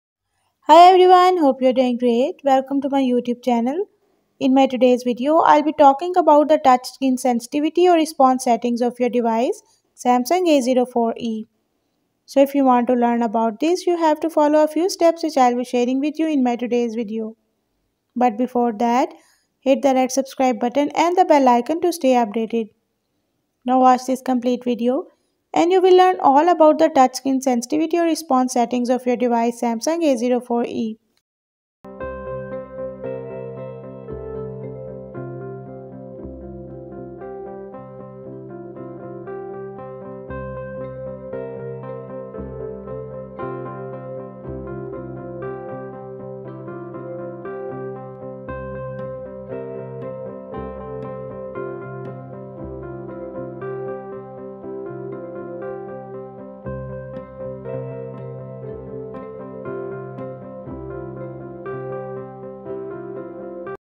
Samsung Galaxy A04e Touch Screen Sensitivity || How to use touch sensitivity setting
0:24 Touch Screen Sensitivity